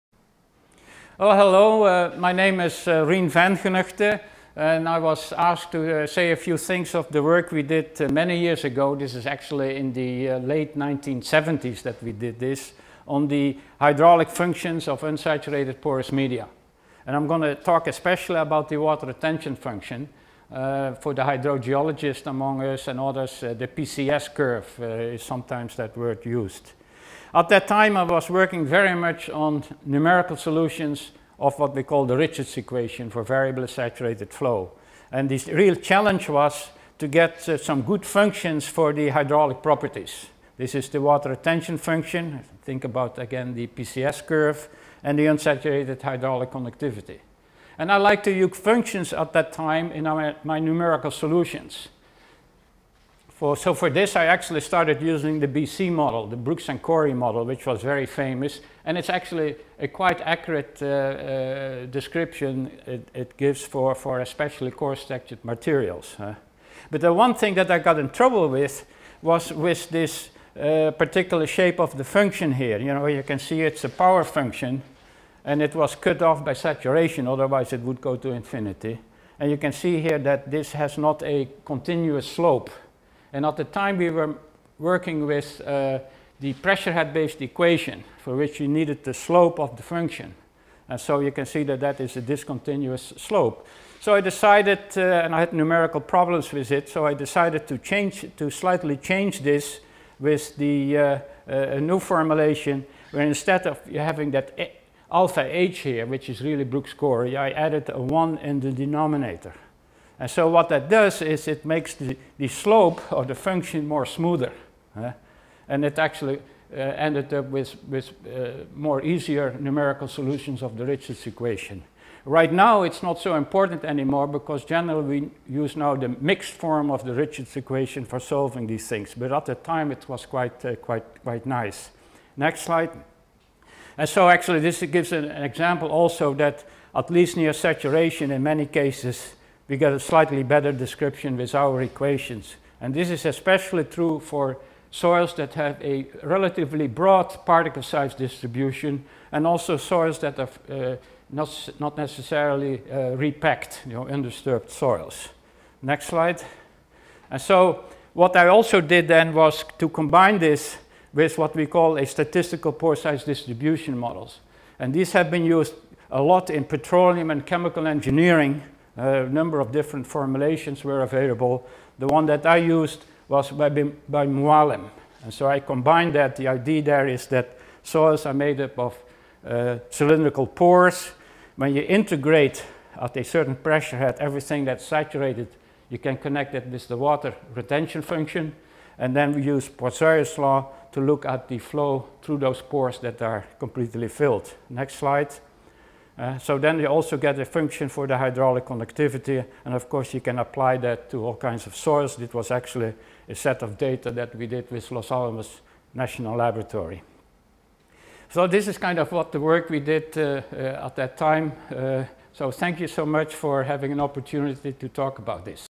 2 5 2 3 1 Rien van Genuchten: Water retention and conductivity functions
Rien van Genuchten presents the development of his water retention and conductivity equations in July 2018 in Cargese France.